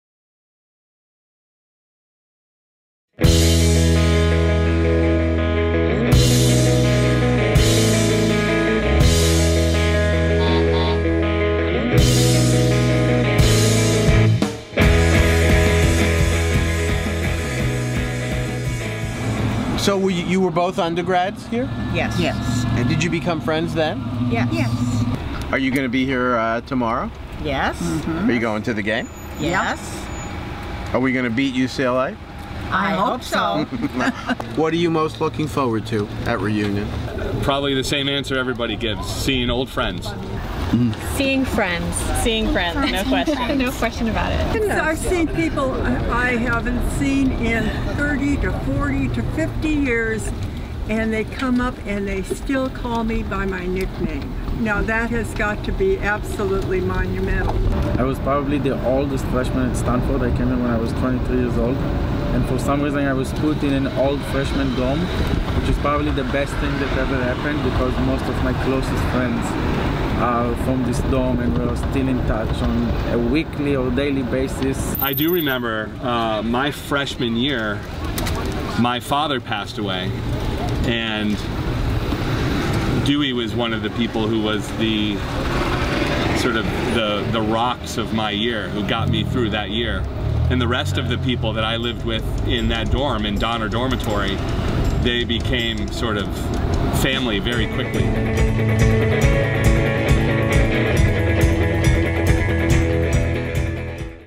Golf Cart Confessions: "Farm friends forever..."
There's something magical about looking back and thinking about how that random guy or girl down the hall turned into the best friend you're hanging out with at Reunion 10, 20, even 50 years later. (Res-Ed sure knows what they're doing!) Back on the Farm for their Reunion, Stanford alumni of all ages tell tales about making forever friends at Stanford.

Starring Michael "Nooch" Antonucci as the chatty cabbie.
Created and produced by Larissa Eisenstein, '00
Shot by Adrian Carey and Larissa Eisenstein, '00
Edited by Adrian Carey
Original music by The Never Setting Suns

Golf Cart Confessions is an original series presented by the Stanford Alumni Association. Shot on location at Stanford Reunion Homecoming, 2013.